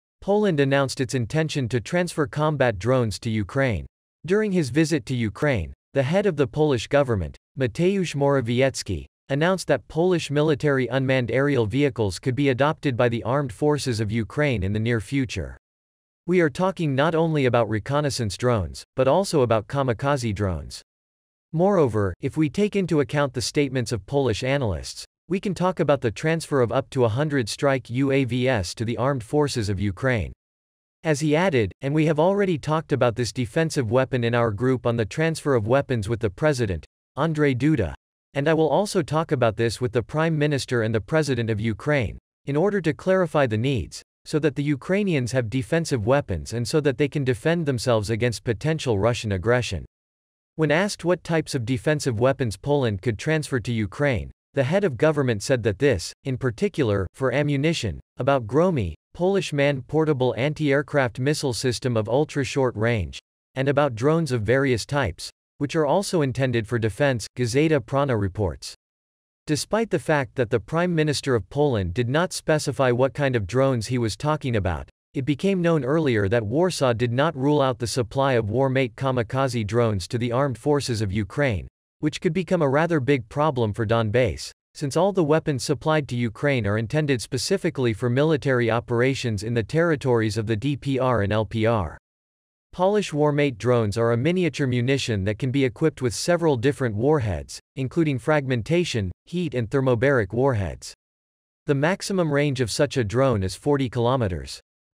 Poland announced its intention to transfer combat drones to Ukraine
During his visit to Ukraine, the head of the Polish government, Mateusz Morawiecki, announced that Polish military unmanned aerial vehicles could be adopted by the Armed Forces of Ukraine in the near future. We are talking not only about reconnaissance drones, but also about kamikaze drones. Moreover, if we take into account the statements of Polish analysts, we can talk about the transfer of up to a hundred strike UAVs to the Armed Forces of Ukraine.
“As he added, “and we have already talked about this defensive weapon in our group on the transfer of weapons with the president (Andrzej Duda) and I will also talk about this with the prime minister and the president of Ukraine, in order to clarify the needs, so that the Ukrainians have defensive weapons and so that they can defend themselves against potential Russian aggression.” When asked what types of defensive weapons Poland could transfer to Ukraine, the head of government said that this, in particular, “for ammunition, about Gromy (Polish man-portable anti-aircraft missile system of ultra-short range) and about drones of various types, which are also intended for defense"", - Gazeta Prawna reports.

Despite the fact that the Prime Minister of Poland did not specify what kind of drones he was talking about, it became known earlier that Warsaw did not rule out the supply of Warmate kamikaze drones to the Armed Forces of Ukraine, which could become a rather big problem for Donbass, since all the weapons supplied to Ukraine are intended specifically for military operations in the territories of the DPR and LPR.

Polish Warmate drones are a miniature munition that can be equipped with several different warheads, including fragmentation, HEAT and thermobaric warheads. The maximum range of such a drone is 40 kilometers.